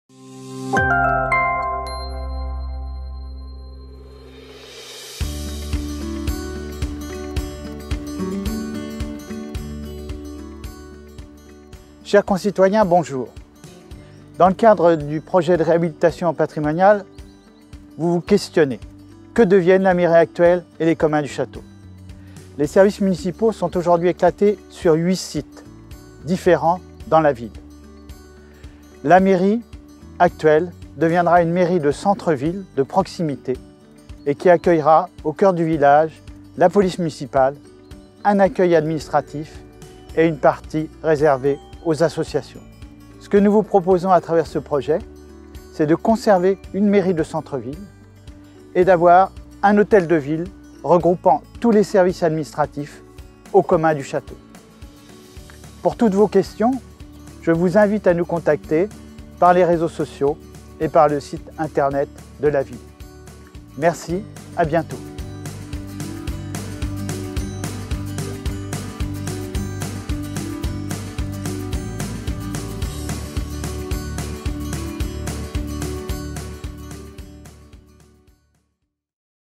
Réhabilitation patrimoniale - n°2 : l'avenir de l'hôtel de ville et de la mairie du centre-ville ?
#2 Regroupement des services 🎬

"Projet de réhabilitation patrimoniale - Question n°2 : "Quel est l'avenir de l'hôtel de ville et de la mairie du centre-ville ?"

Vous vous interrogez sur l'origine du projet de réhabilitation du patrimoine ? Henri de Meyrignac, maire de Vaux-le-Pénil, répond à vos questions 📣

🔸Les services municipaux sont aujourd'hui éclatés sur 8 sites différents dans la ville. La mairie actuelle deviendra une mairie de centre-ville de proximité et qui accueillera au cœur du village la police municipale, un accueil administratif et une partie réservée aux associations. Ce projet vous propose de conserver une mairie de centre-ville et d'avoir un hôtel de ville regroupant tous les services administratifs aux Communs du Château.

N'hésitez pas à nous transmettre vos interrogations via cette page Facebook ou sur le site Internet rubrique "Nous contacter" 💬

➡️ Rendez-vous jeudi prochain pour la question n°3 : "Que vont devenir les associations pendant la durée des travaux?"

➡️Vous pouvez également consulter l'étude de stratégie patrimoniale sur la ville de Vaux-le-Pénil